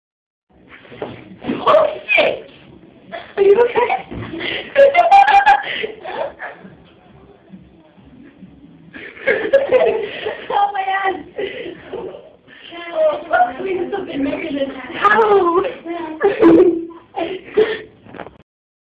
Dressing Room Disaster- S&L
Seanie spilled a big bottle of lotion in the dressing room causing her to sleep and Lexie tried to help her but that didn't work.